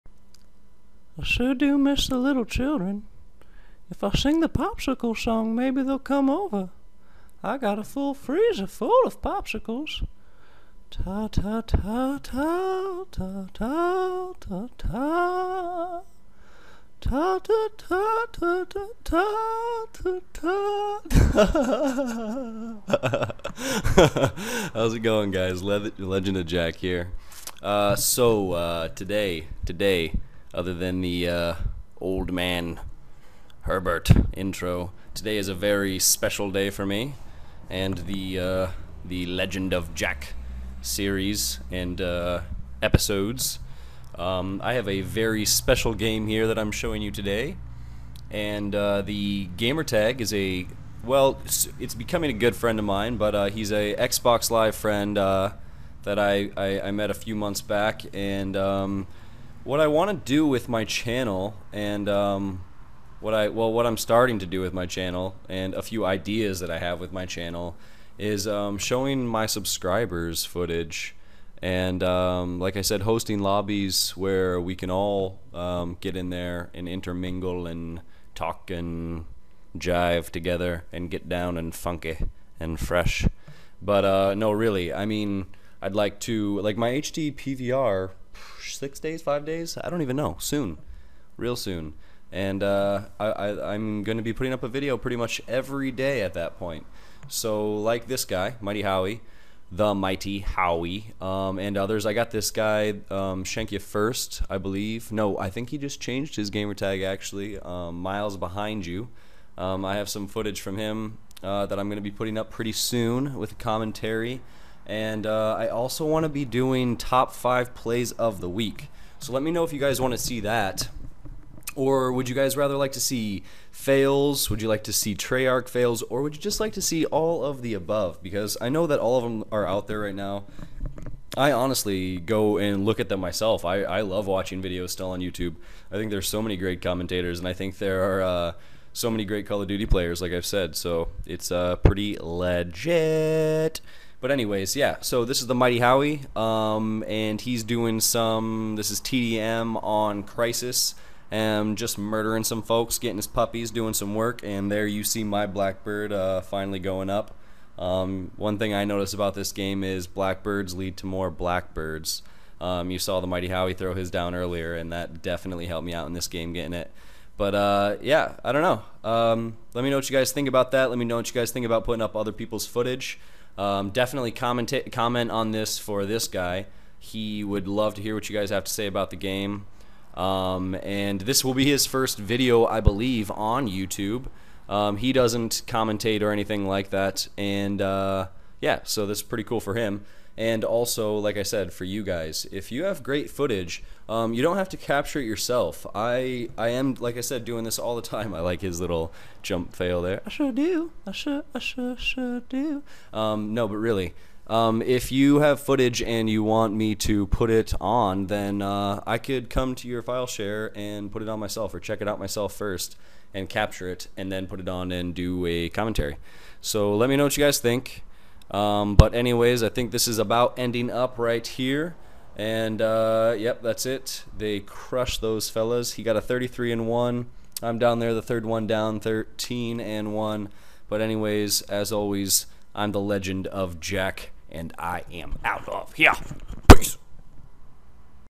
Legend of Jack presents.....The Mighty Howie meets Old Man Herbert!
Judge me sparingly on the impression...i just kinda let it flow!

lol as always comment, and rate

shout at my boy The Mighty Howie and tell him what you think of his game!